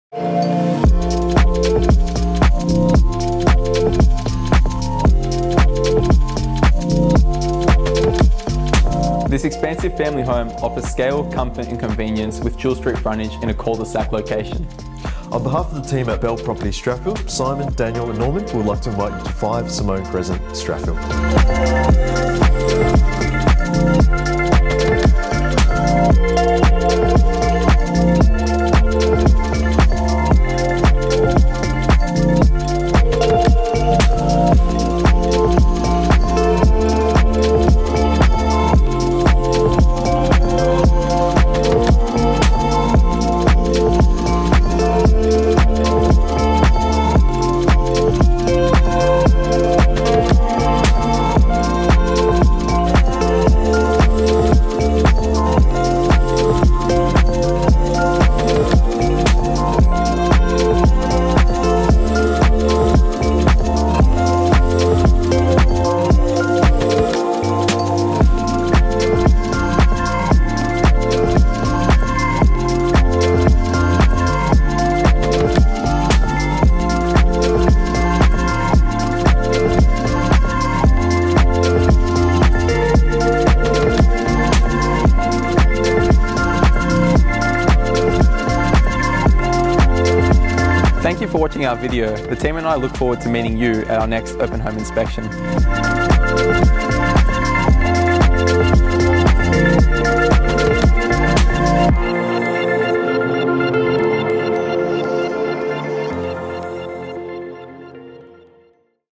[Belle Property Strathfield] 5 Simone Crescent, Strathfield_V2
Impressive scale, comfort and convenience in cul-de-sac location with dual street frontage

Nestled on a generous 727sqm parcel in an idyllic cul-de-sac locale, this expansive family home provides a contemporary move-in ready option of impressive scale, comfort and convenience. Its light filled, north facing layout, showcases a selection of living areas that spill out to an alfresco area for effortless indoor/outdoor entertaining. Set at the end of a quiet family friendly street, its location is a short walk to the best of Homebush and Strathfield amenities.

- Selection of formal and casual living areas, fireplaces, polished floors
- Bright north facing aspect allows abundant natural light

- At the end of a cul-de-sac, whisper quiet family friendly locale

- Timber panelled walls, large retro timber kitchen, meals area

- Five large bedrooms, three stylish bathrooms, parking for 4 cars

- Rare dual street frontage to Mackenzie Street, large 727sqm parcel

- Landscaped front and rear gardens, potential for secondary home STCA

- Walk to village shops, Homebush train, local schools and parks

- Stroll to Strathfield train, the Plaza, Boulevarde shops and retail